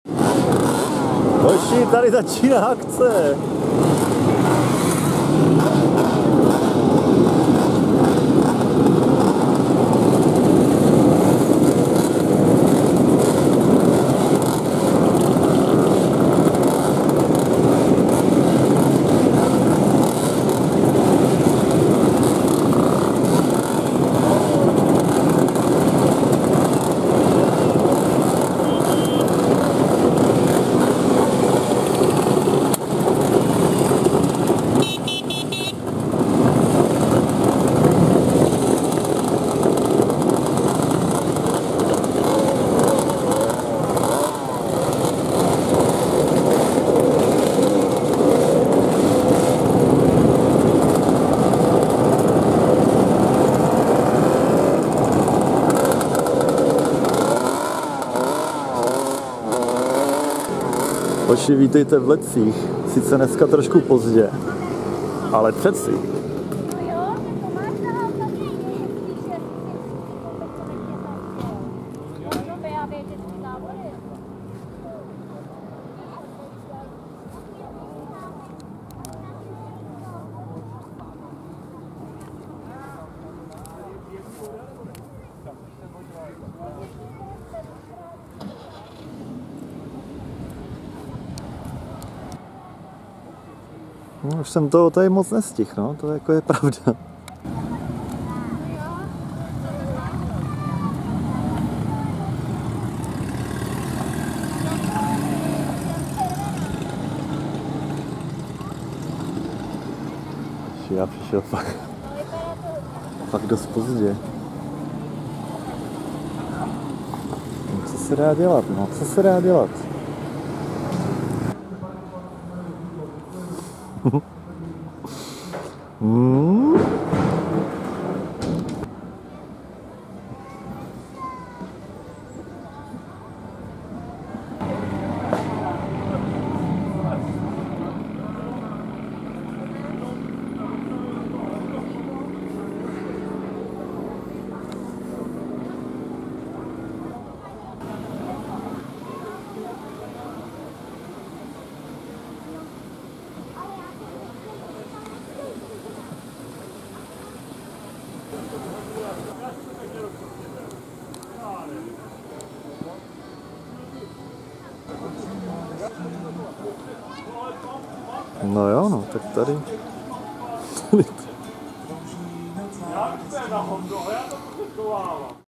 NESTIHL JSEM Sraz přátel značky Jawa 2020 ► by Berny
Vůně spáleného benzínu, řev motorů péráků, kývaček nebo panelek a spousta další podívané už po páté v rodišti  zakladatele značky Jawa, pana Františka Janečka. Akce je určena také pro historické automobily a motocykly jiných značek. No a já se tam vydal v sobotu 5.9.2020 bohužel letos pozdě...